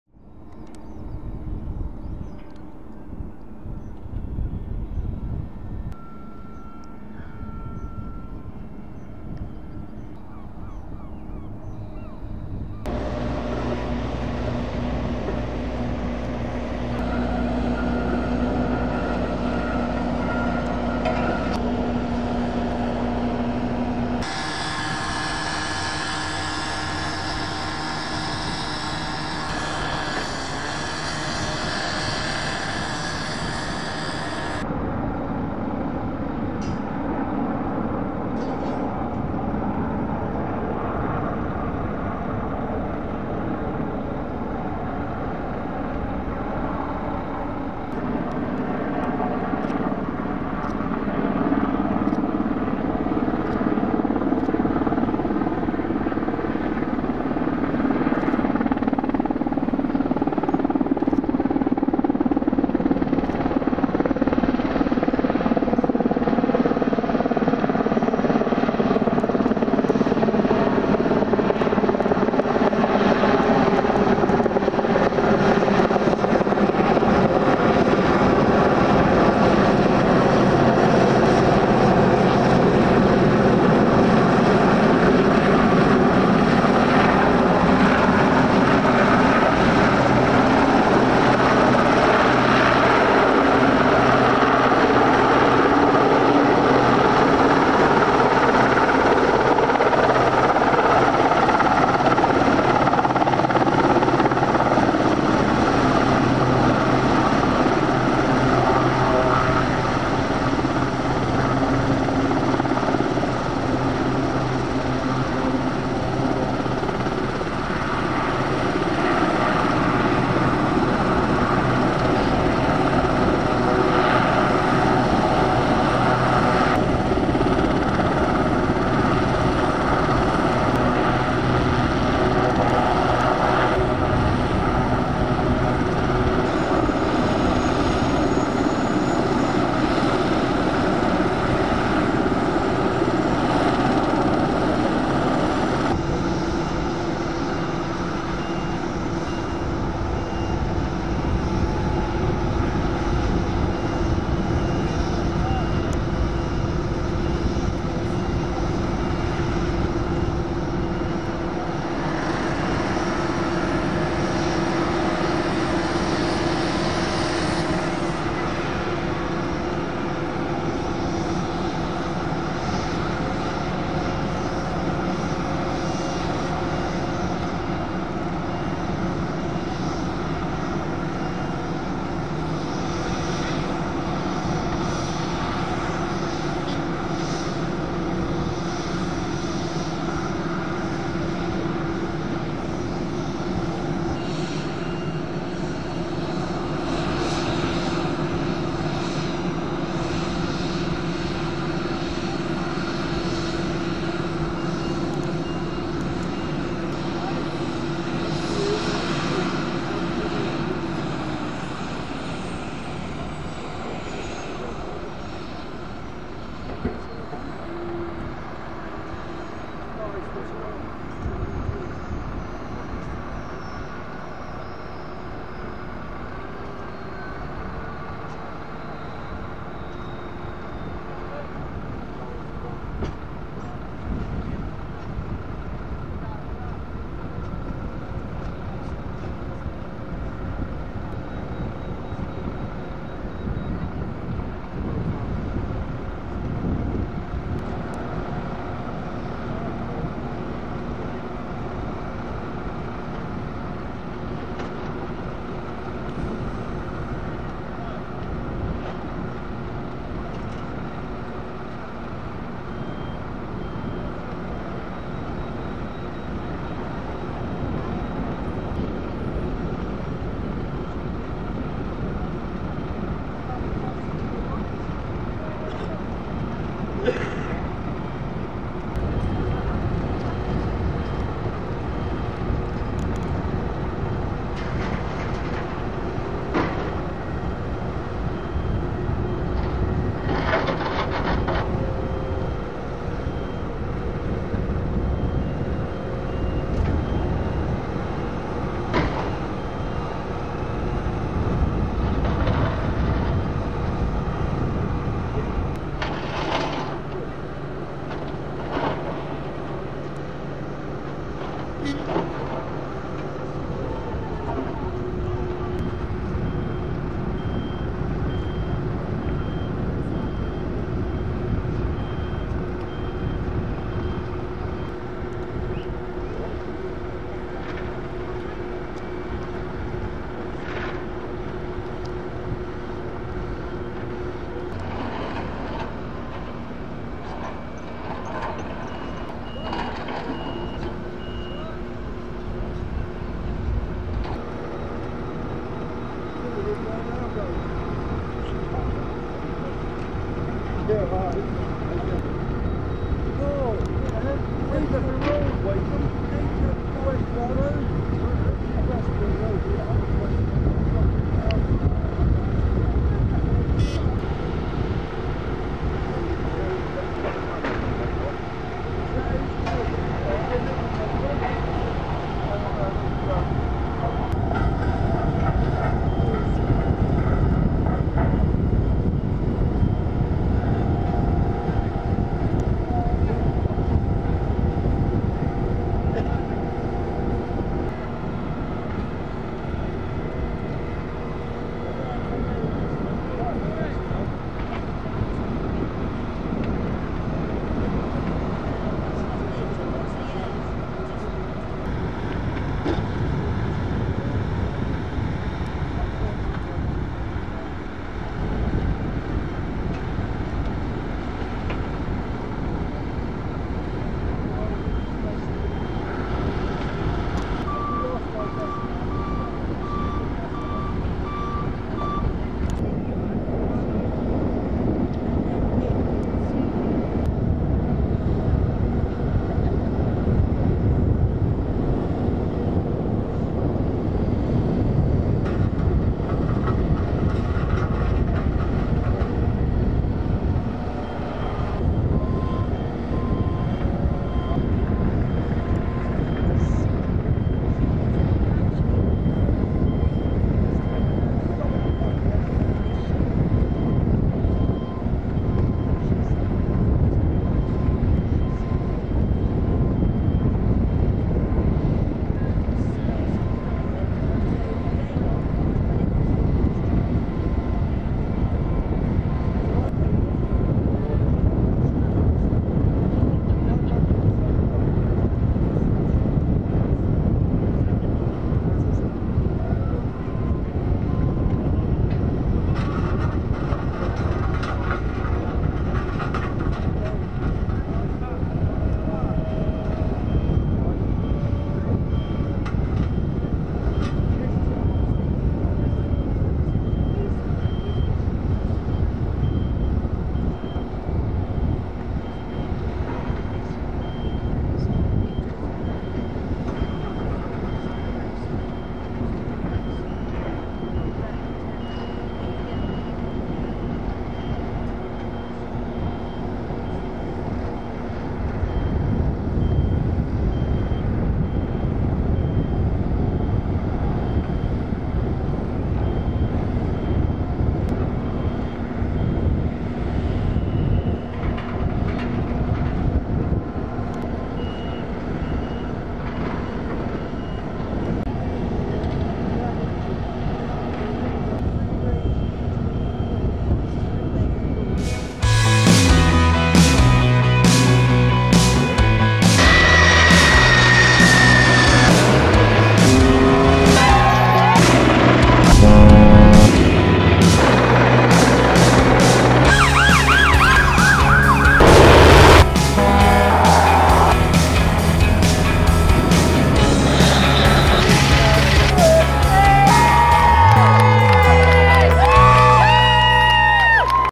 Sea King helicopter returns as runway is resurfaced 🚁 🚧
As the runway at Portland Heli-Operations in Dorset is resurfaced, a Sea King helicopter returns to base. The Sea King, formerly owned by the Royal Navy, is used by HeliOps to provide training to military and emergency services.

The base, formerly known as RNAS Portland (HMS Osprey), also provides other services such as fuel to military helicopters. Currently the runway is being resurfaced and therefore there are limited visitors, usually you would have CV-22 Ospreys from the US Air Force, Merlins from the Royal Navy and more landing here. Some of the Sea Kings are being repurposed for use in Ukraine.

Ⓒ CobraEmergency